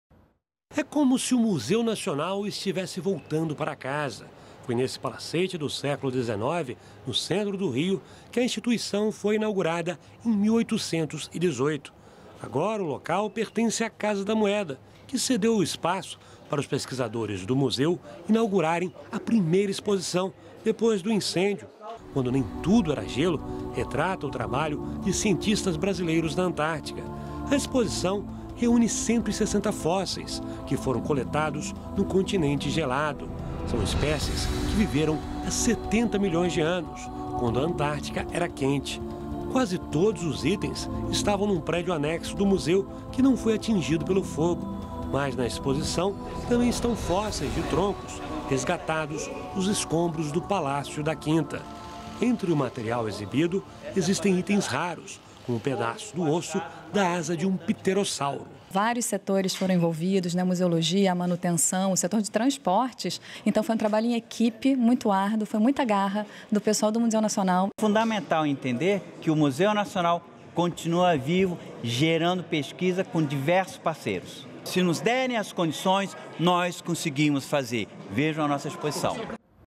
Museu Nacional organiza primeira exposição após incêndio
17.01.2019 - Museu Nacional inaugura a exposição "Quando nem tudo era gelo - Novas descobertas no continente Antártico".